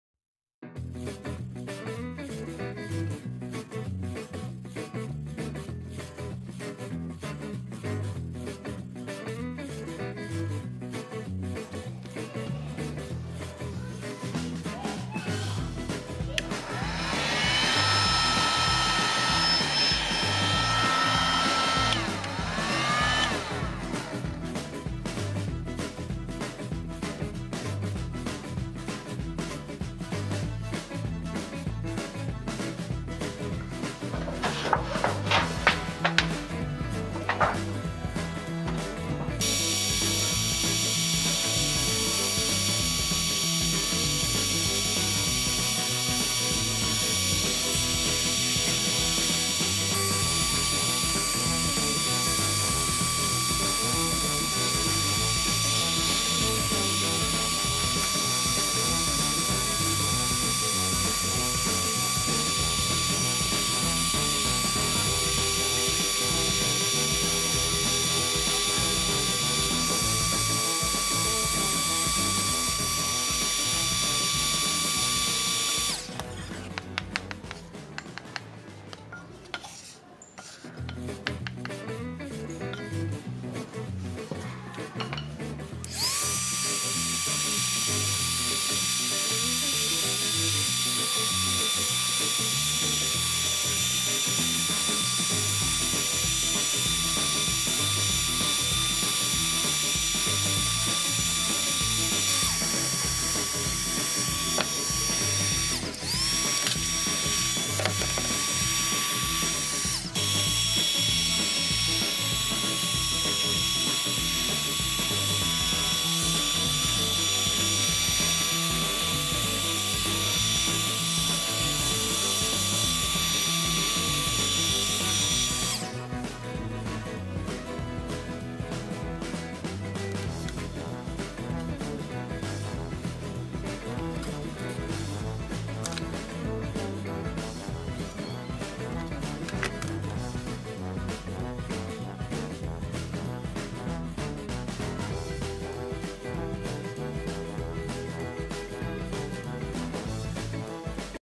Como Proteger utensilios de madera
La humedad y el agua deterioran rapidamente estos articulos de madera para esto hay que impregnarlos de cera a base de calor